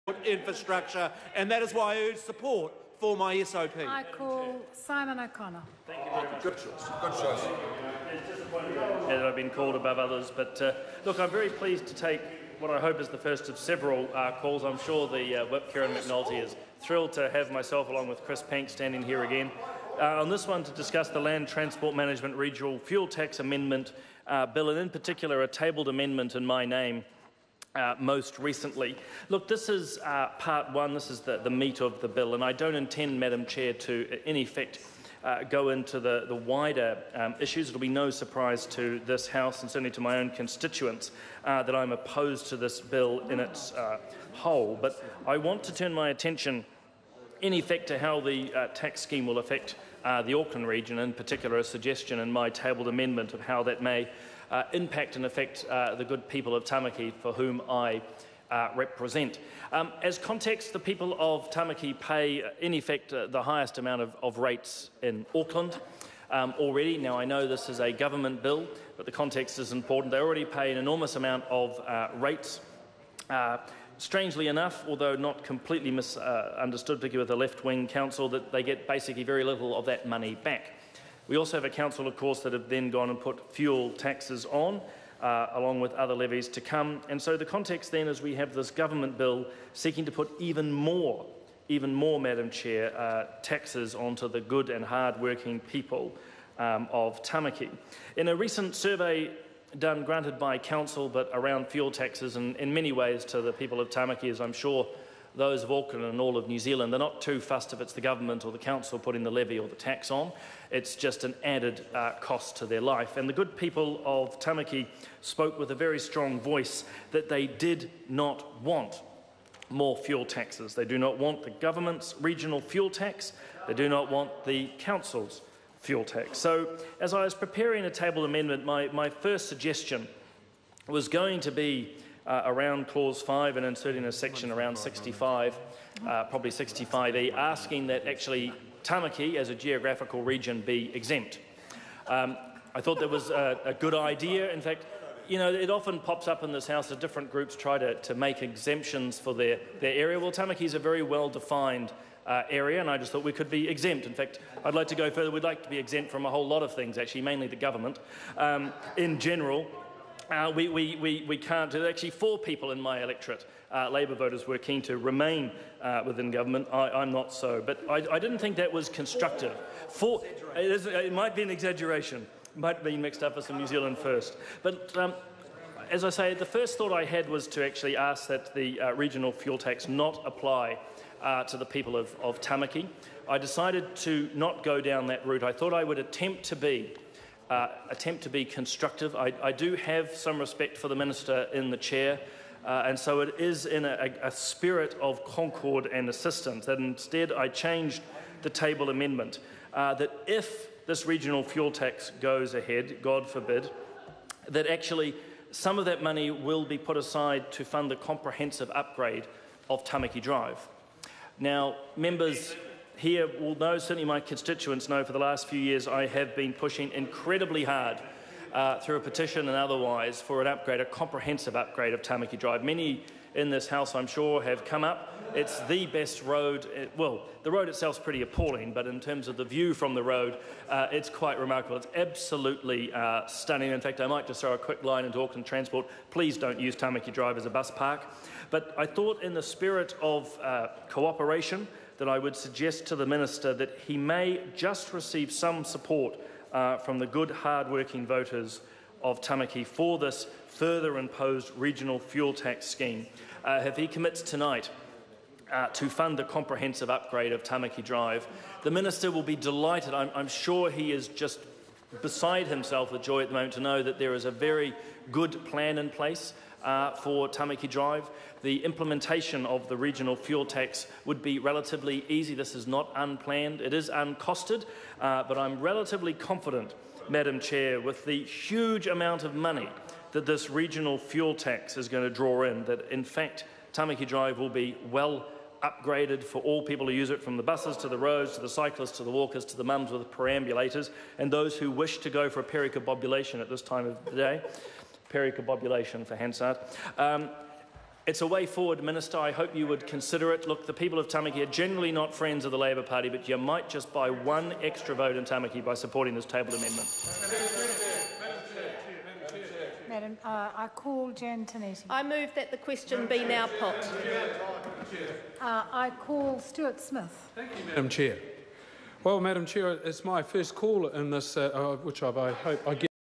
Land Transport Management (Regional Fuel Tax) Amendment Bill - Committee Stage - Part 1 - Video 38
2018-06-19 - Land Transport Management (Regional Fuel Tax) Amendment Bill - Committee Stage - Part 1 - Video 38

Simon O'Connor